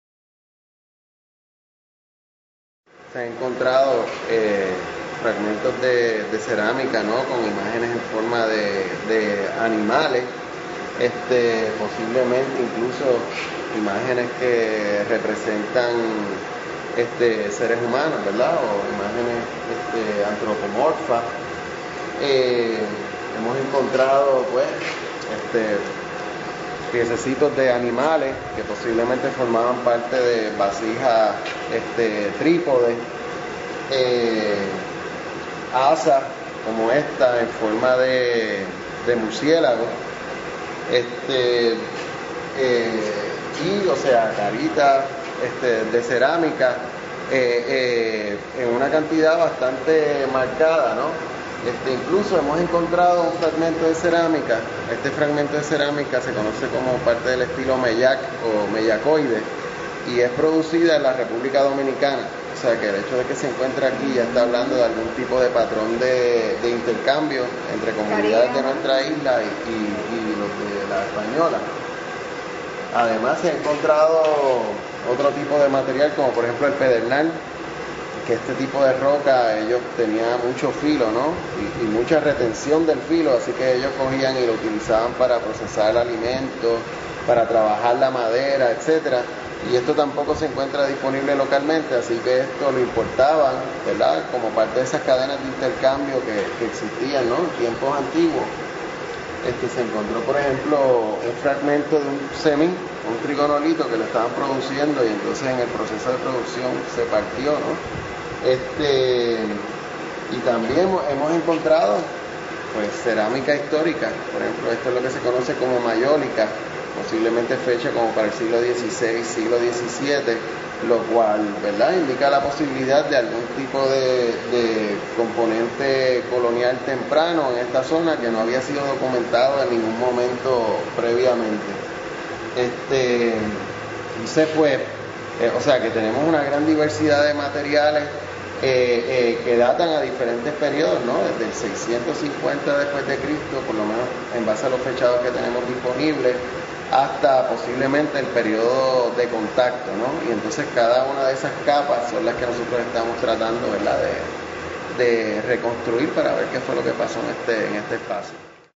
Valiosos hallazgos arqueológicos en Utuado
Las excavaciones arqueológicas que comenzaron como parte de un curso de campo en la Universidad de Puerto Rico en Utuado (UPRU) han resultado en importantes hallazgos de diferentes componentes culturales, que pudiesen ser tan remotos como de 650 años